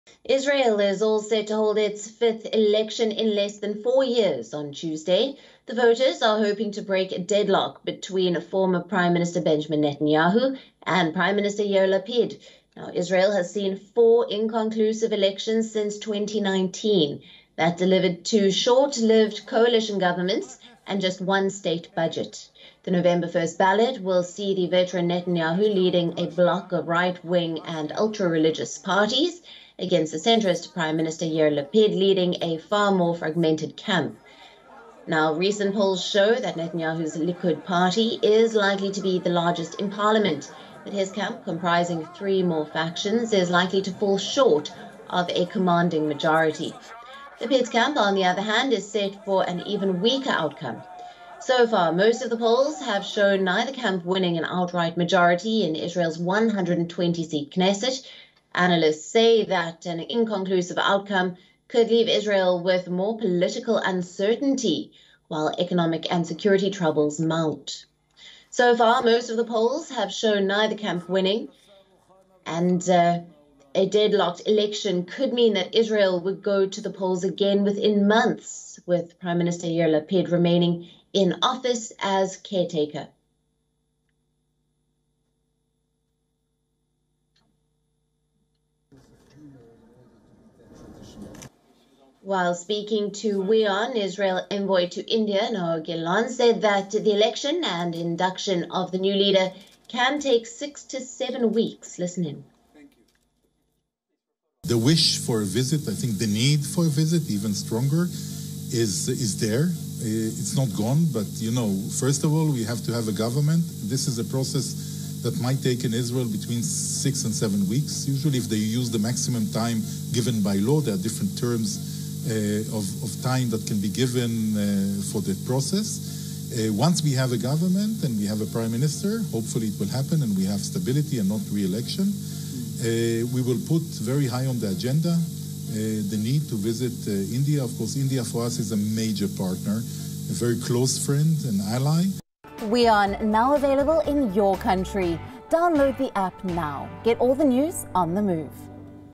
Israel to hold fifth election in four years: It's Benjamin Netanyahu vs Yair Lapid | Top World News
Israel is all set to hold its fifth elections in less than four years on Tuesday hoping to break the deadlock between former Prime Minister Benjamin Netanyahu and Yair Lapid. Israel has seen four inconclusive elections since 2019 that delivered two short lived coalition governments.